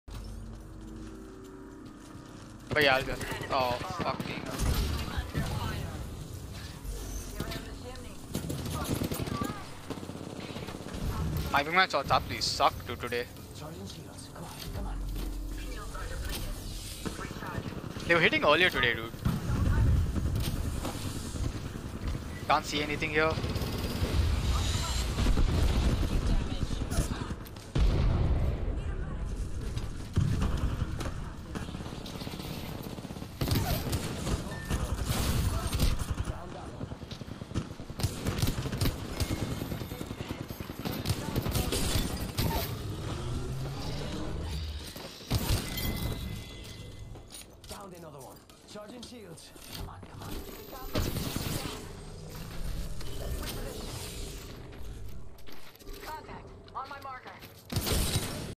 A bit of some WINGMAN ACTION !!
Teammates dead - Solo win in Ranked Cont. !!

- if you like the content drop a sub on YT and comment or a follow on Twitch!!